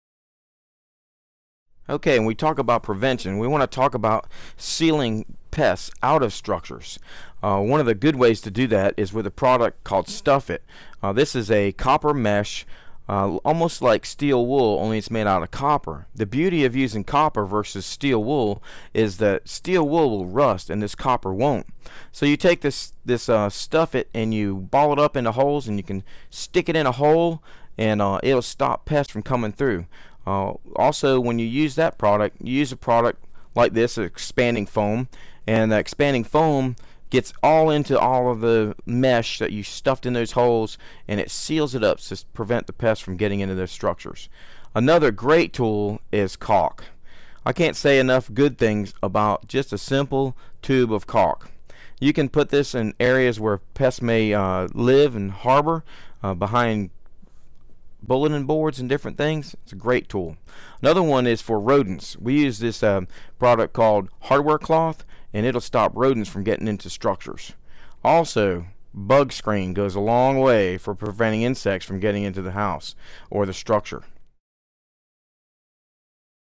Prevention: Pest Control IPM Step 4
Prevent pests from moving into a building. Create a barrier that keeps them out by using these IPM methods. Fill in the gaps, holes and cracks with helpful products.